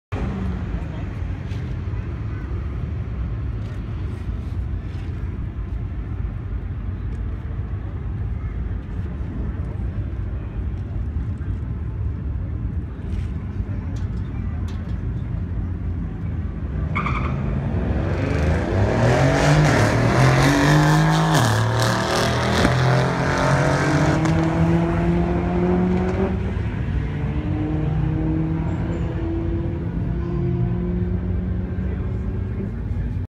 VW 1.8t 2003 Jetta Wolfsburg frankenturbo vs Twin turbo BMW
Getting killed by a BMW